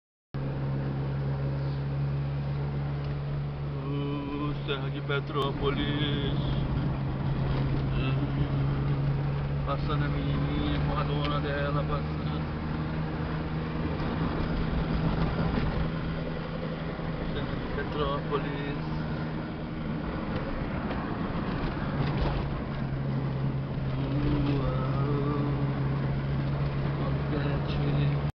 SERRA DE PETRÓPOLIS/RIO DE JANEIRO/VOLTANDO DAS PRAIS DE CABO FRIO/PARA BELO HORIZONTE/MINAS GERAIS
VOLTANDO DAS PRAIAS DE CABO FRIO/RIO DE JANEIRO/BRASIL